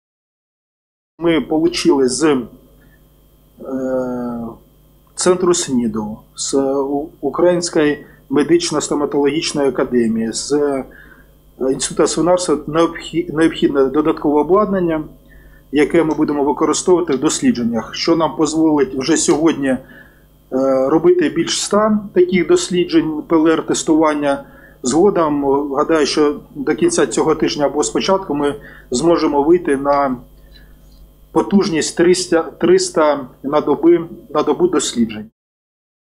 Дмитро Лунін: ремонт в обласній лабораторії вже завершено
Обладнання для другої лабораторії, в якій робитимуть тести на коронавірус, почали встановлювати в обласному лабораторному центрі. Це – ламінарна шафа для роботи вірусолога та спеціальна центрифуга. Також очікують на поставку станції підготовки проб. Це обладнання у тимчасове користування надали Українська медична стоматологічна академія та обласний «Центр профілактики ВІЛ-інфекції»